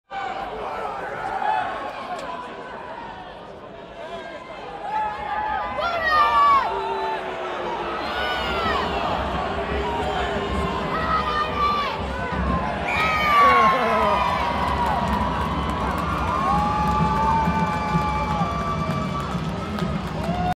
London Irish 20 second try from a line out
11/012014 London Irish v Lusitanos XV final score 79-3